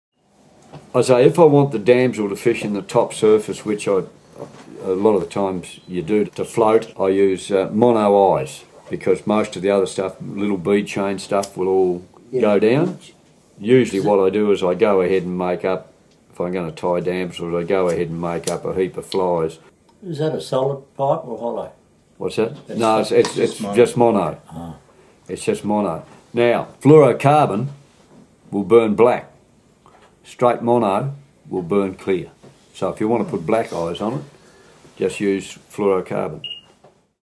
Making Damsel Nymph Eyes
Brian McCullagh became hooked on fly fishing in the early 60’s, whilst living as an apprentice in the central highlands of Tasmania. He has been a member of the Tasmanian Fly Tyers Club since 1983 and started guiding in the early 90’s whilst teaching fly tying at nights. He won his first state championship in 1994 and went on to captain and manage the Australian Fly Fishing teams in Finland and New Zealand. He still loves his fishing and manages to squeeze in an annual foray into New Zealand for two to three weeks a year and Northern Australia chasing barramundi.

Here he demonstrates how to make dumbell eyes for a damsel nymph pattern that will allow the fly to be fished higher in the water column by melting 60lb monofilament. Check out Brian's video on tying his damsel nymph